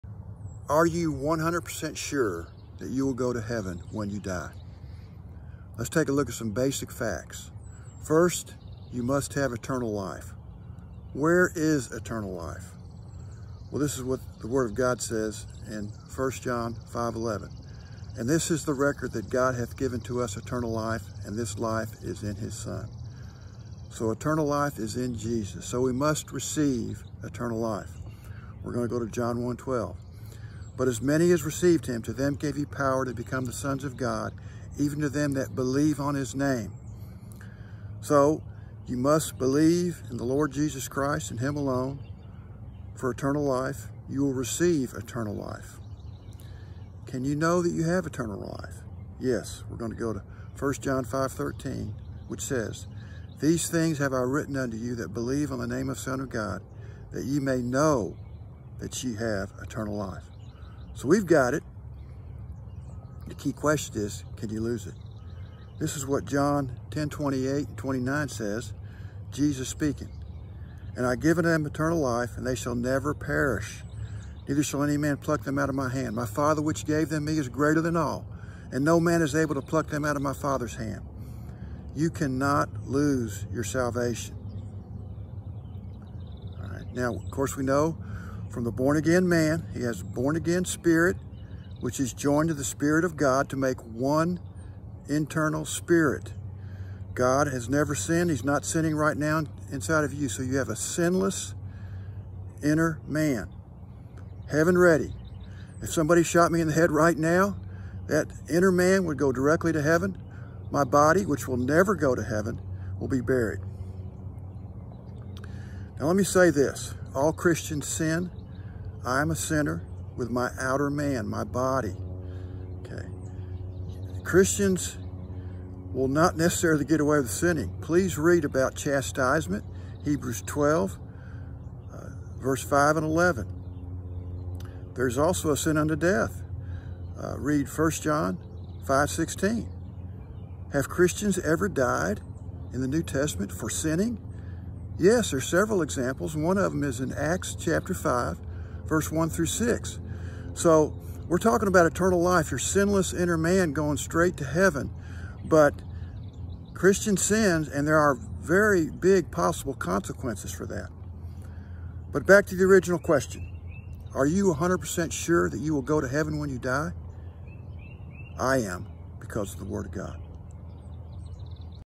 8 - Are you 100% Sure You Will Go to Heaven When You Die
This video explains how you can be 100% sure You Will Go to Heaven When You Die